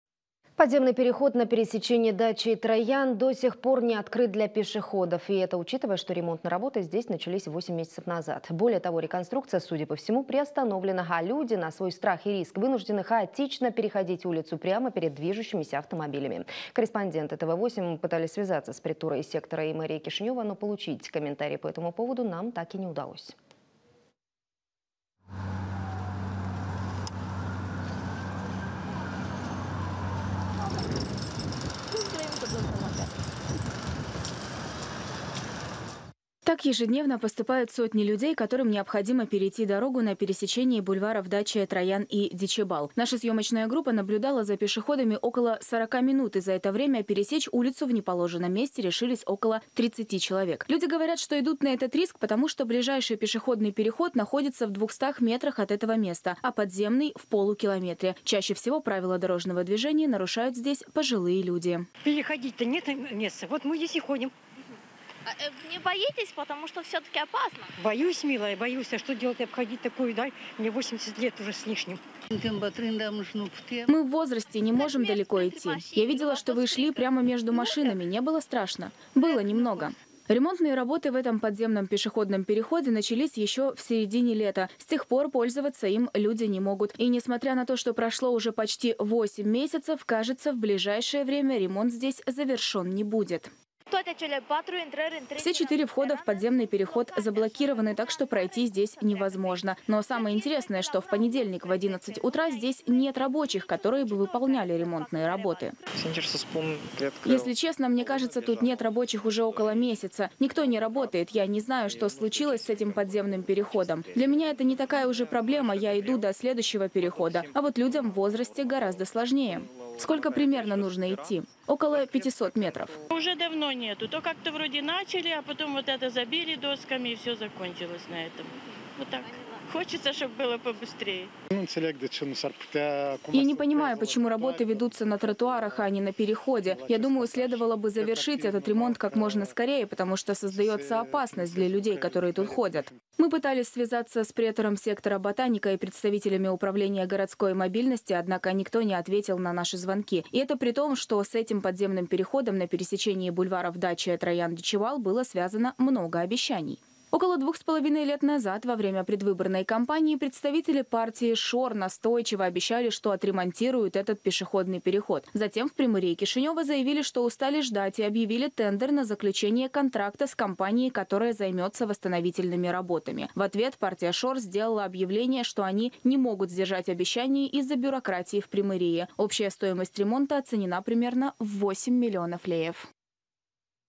Работы без работников. Ремонт подземного перехода не продвигается уже 8 месяцев
Подземный переход на пересечении проспектов Дачия и Траян, до сих пор не открыт для пешеходов. И это, учитывая то, что ремонтные работы здесь начались 8 месяцев назад Более того, реконструкция, судя по всему, приостановлена, а люди, рискуя жизнями, вынуждены хаотично переходить улицу прямо перед движущимися автомобилями. Корреспонденты ТВ8 попытались связаться с претурой сектора и мэрией Кишинева, но получить комментарии по этому поводу нам так и не удалось.